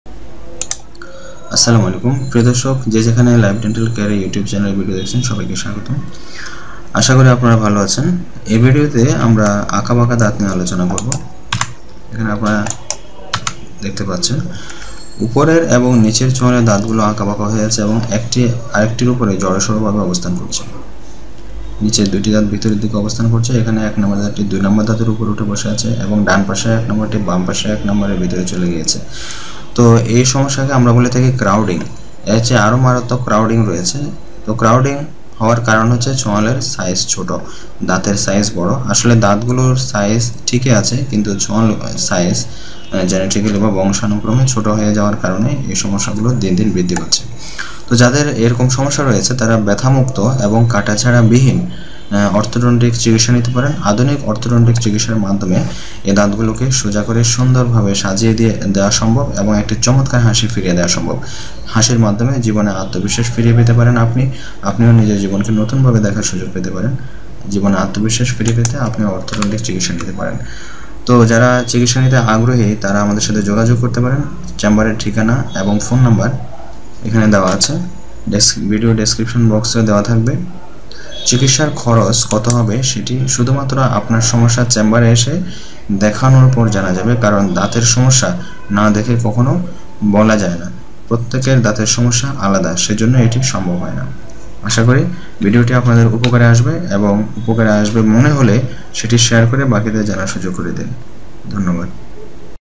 আঁকা বাঁকা দাঁতের চিকিৎসা। লাইফ ডেন্টাল কেয়ার। Dental Crowding treatment.
আঁকা-বাঁকা উঁচু-নিচু ও ফাঁকা দাঁতের চিকিৎসা নিতে পারেন। ব্যাথা মুক্ত ও বিনা অপারেশনে। দাঁত না কেটেই আধুনিক অর্থোডোন্টিক চিকিৎসার মাধ্যমে দাঁতগুলোকে সাজিয়ে সন্দুর ও উজ্জ্বল হাসি উপহার দেয়া সম্ভব। জীবনে নতুনভাবে হাসিতে আত্মবিশ্বাস ফিরে পেতে অর্থোডোন্টিক চিকিৎসার বিকল্প নাই।
যোগাযোগ:
লাইফ ডেন্টাল কেয়ার, বাসা-4, রোড-4, মিরপুর-10 গোলচত্তর, ঢাকা-1216।
মোবা: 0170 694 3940, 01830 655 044